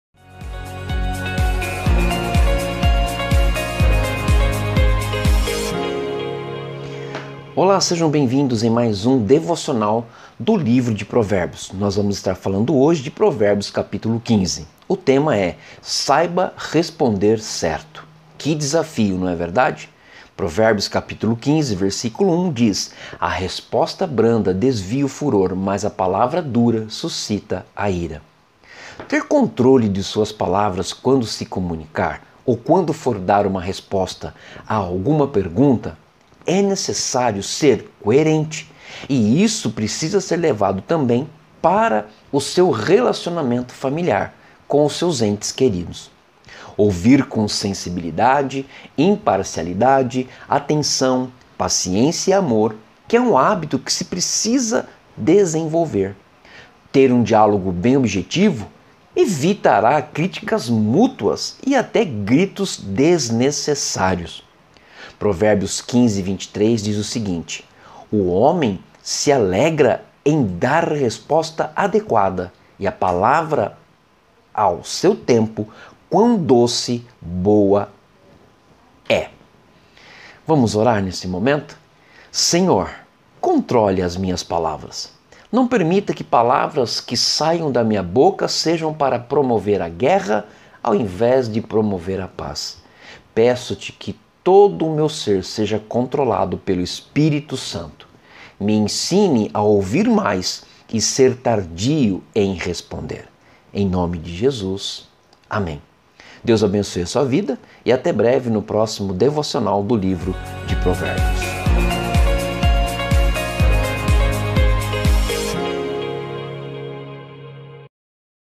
Como aprender dar respostar certas e sábias. Provérbios 15
Provérbios é um livro de sabedoria pratica. Ensina que a religião está ligada aos problemas comuns da vida.

Tema: Saiba responder certo. Provérbios 15:1

“A resposta branda desvia o furor, mas a palavra dura suscita a ira.”

Ação: Ter controle de suas palavras quando se comunicar ou quando for dar uma resposta a alguma pergunta sendo coerente inclusive no relacionamento familiar.

Resultados: Ouvir com sensibilidade, imparcialidade, atenção, paciência e amor que é um hábito que se precisa desenvolver. Ter um diálogo bem objetivo evitando críticas mutuas e até gritos Provérbios 15:23

“O homem se alegra em dar resposta adequada, e a palavra, a seu tempo, quão doce boa é!”

Oração: Senhor, controle as minhas palavras. Não permita que palavras que saiam da minha boca sejam para promover a guerra ao invés da paz. Peço-te que todo o meu ser seja controlado pelo Espírito Santo. Me ensine a ouvir mais e ser tardio em responder. Em nome de Jesus, amém!

👍 Gostou deste video? Deixe um "LIKE"